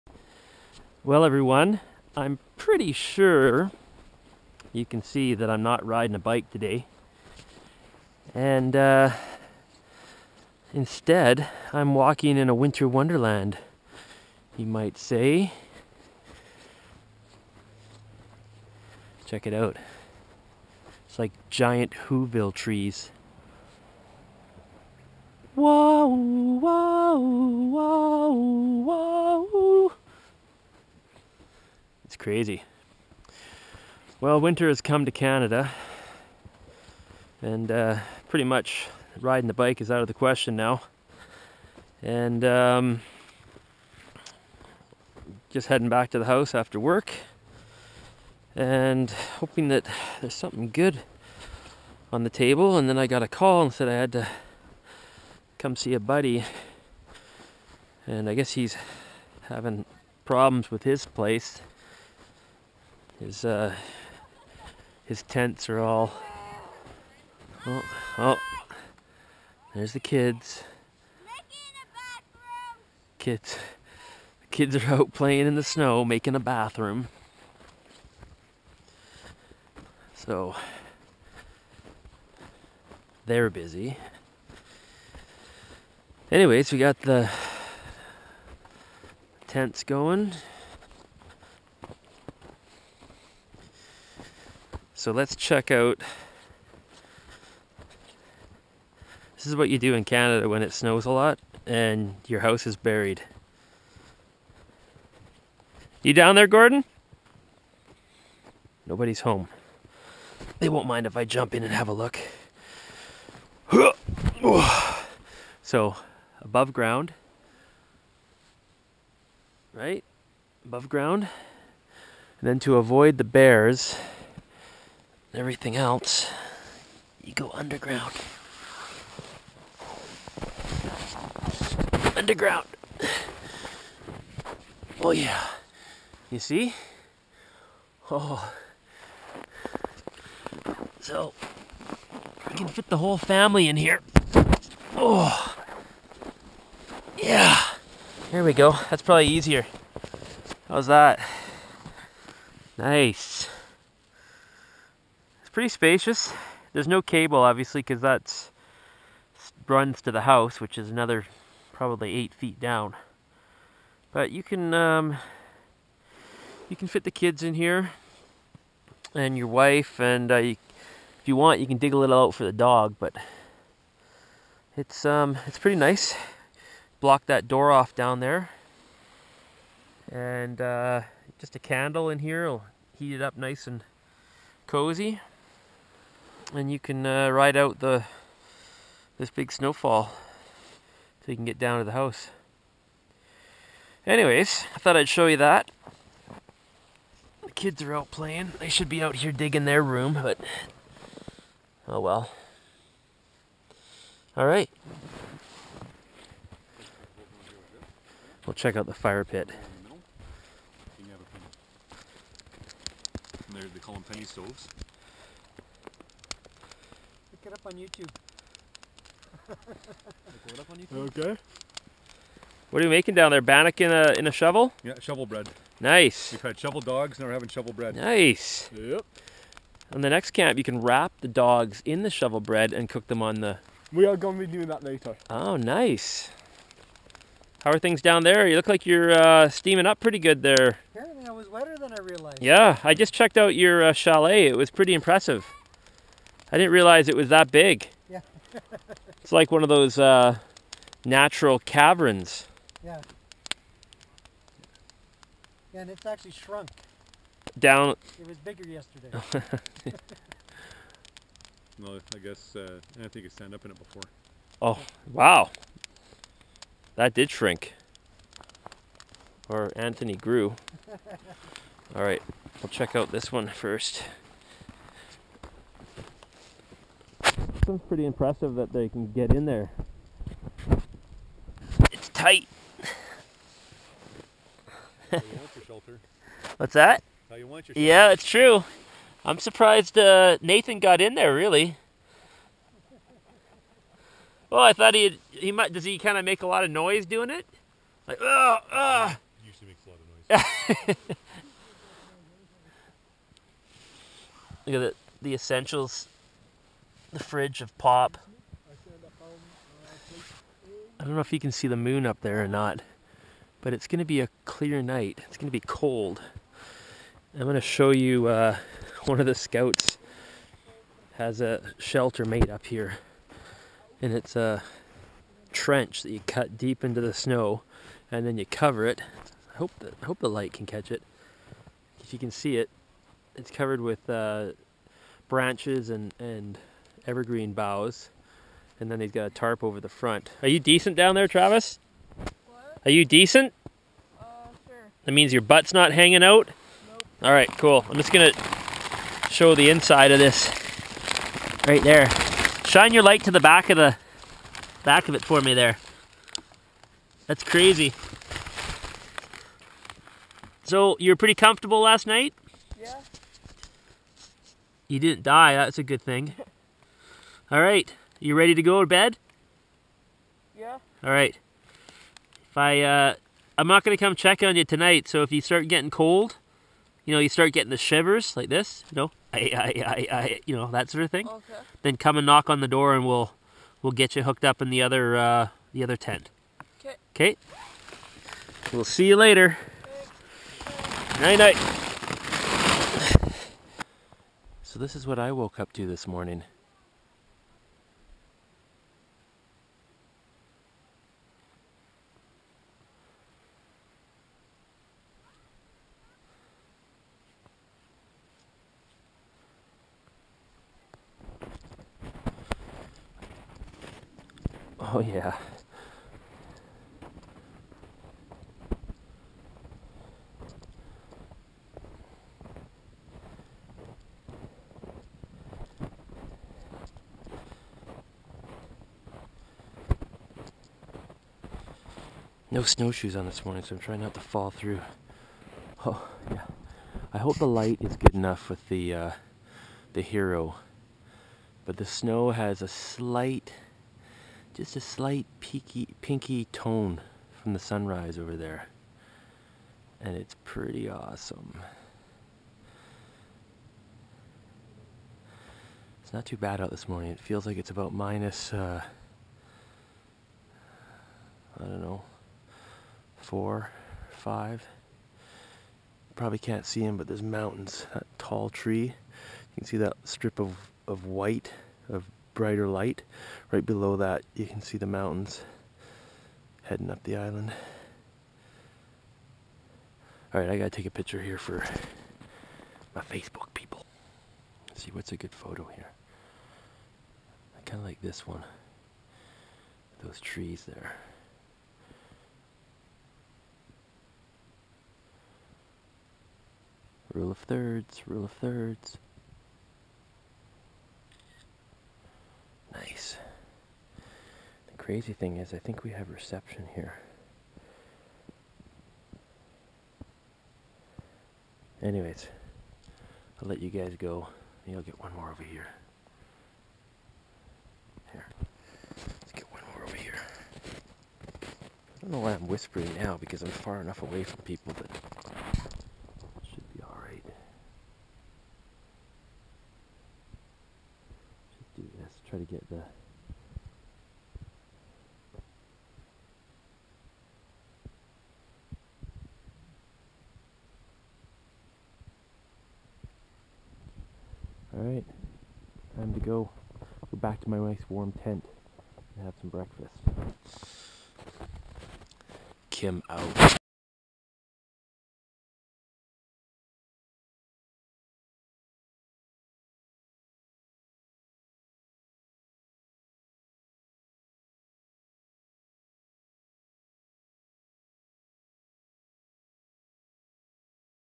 Walk 1: *Winter In Canada*Into The Snow Cave*12 Foot Fire Pit*Trench Shelters*
Hey all you warm weather people...this is what winter in Canada looks like ! The sled dogs were out with the ladies in town so maybe next time you can meet them, but for now check out our street.